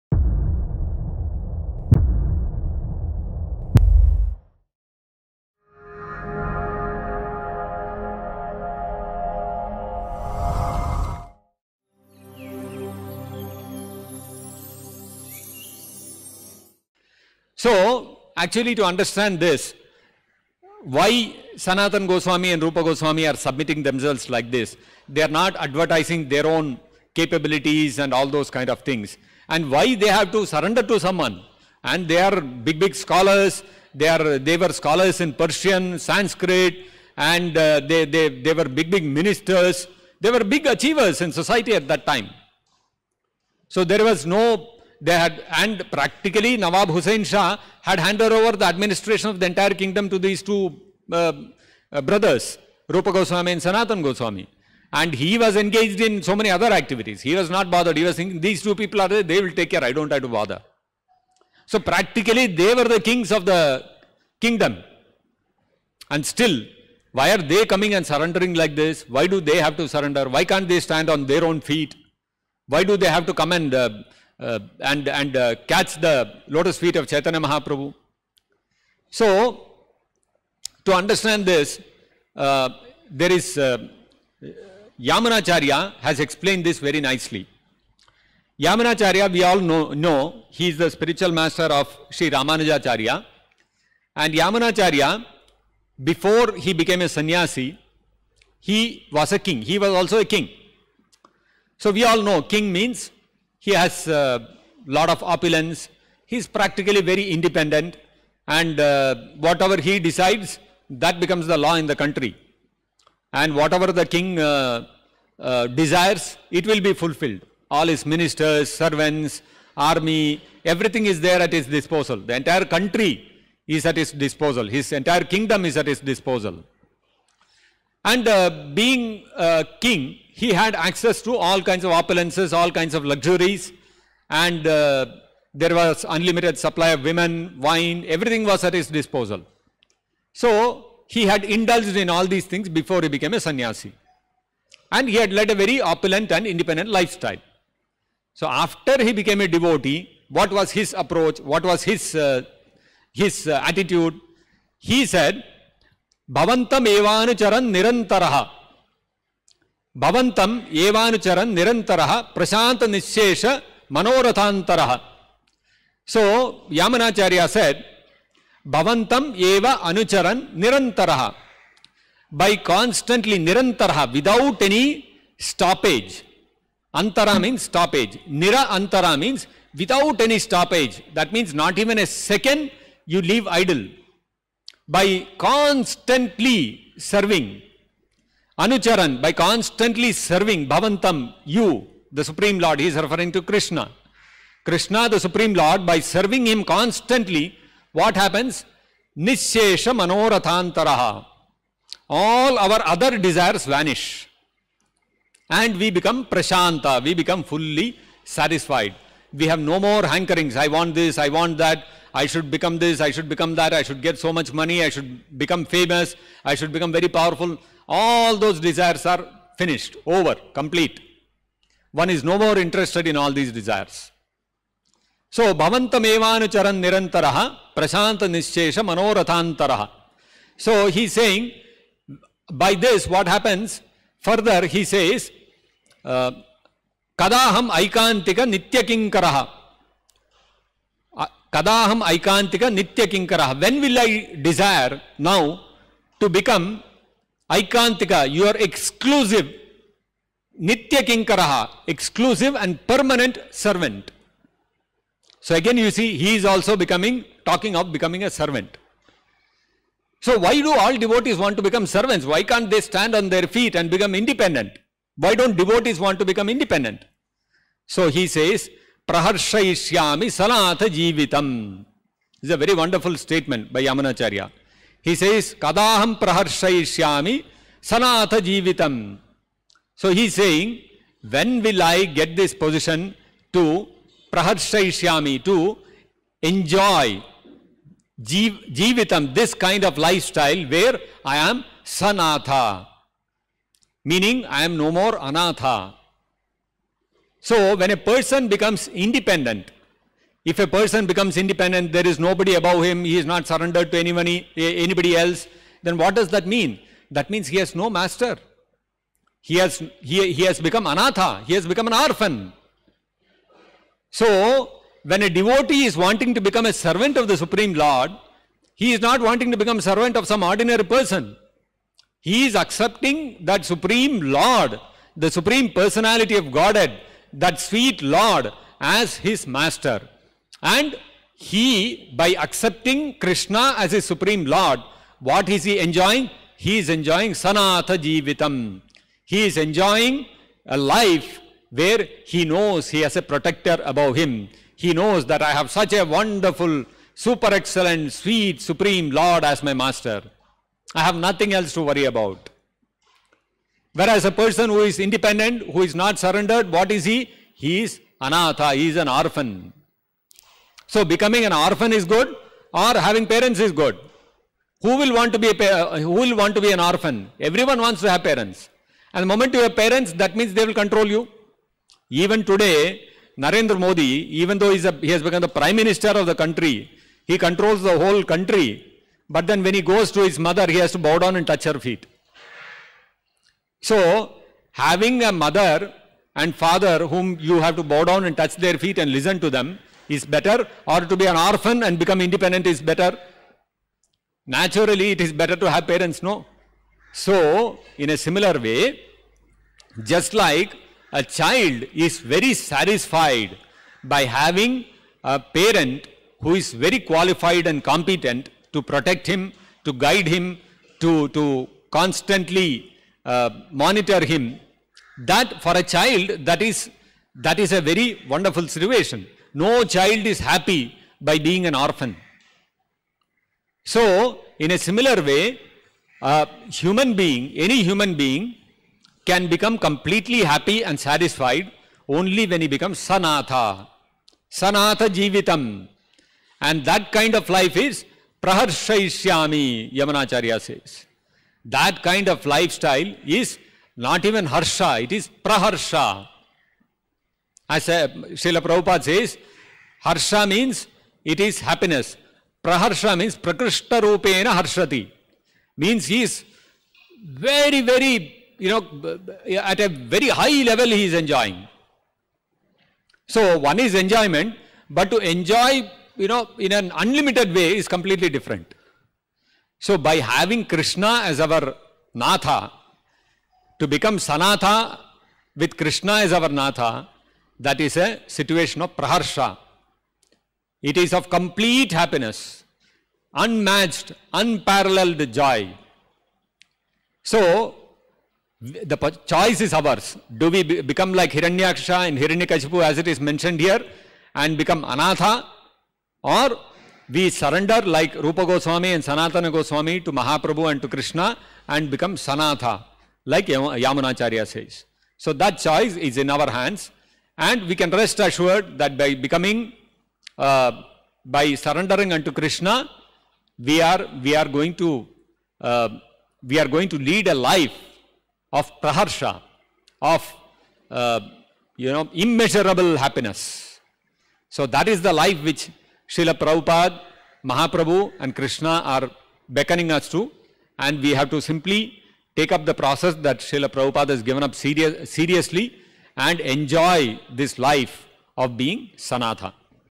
Why do the devotees reject independence? | HG Suvyakta Narasimha Dasa
Excerpts from Srimad Bhagavatam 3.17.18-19 lecture